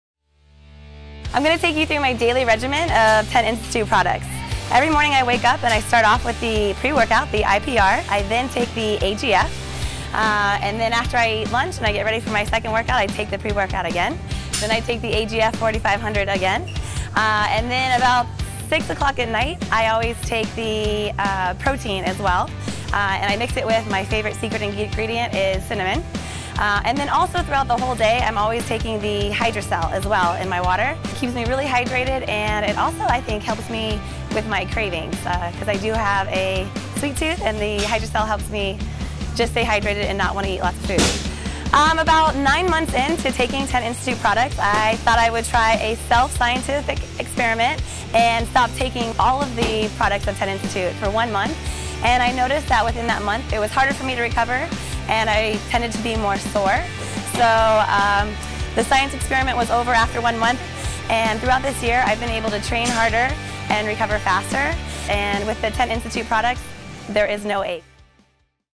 TEN Athlete & Trainer | Jessica Carillon
Jessica Carillon has been coaching Crossfit since May 2012 and coaching Gymnastics since 1998.